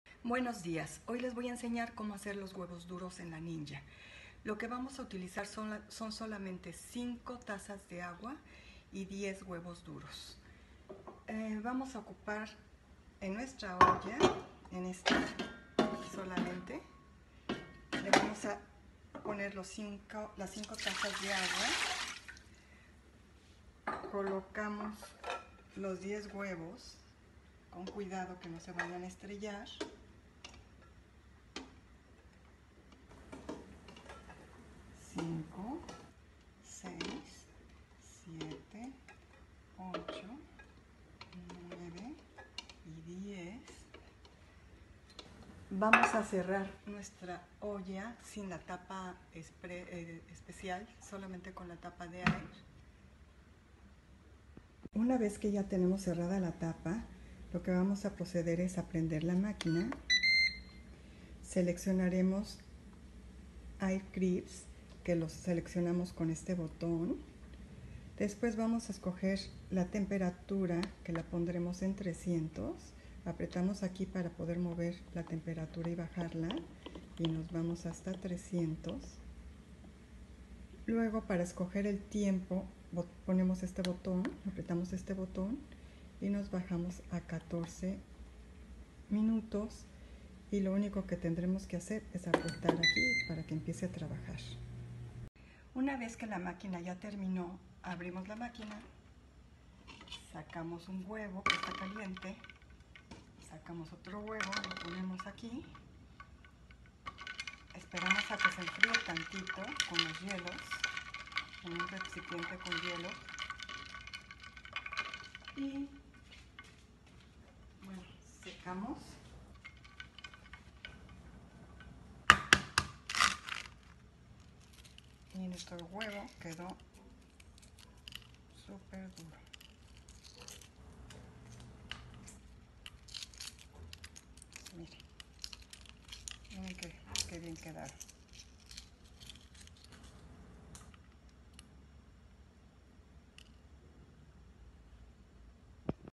huevos* duros con NinjaFoodi*
La mejor técnica para tener unos huevos cocidos perfectos. De esta manera no volverás a tener dificultades para poder incluir en tus platillos.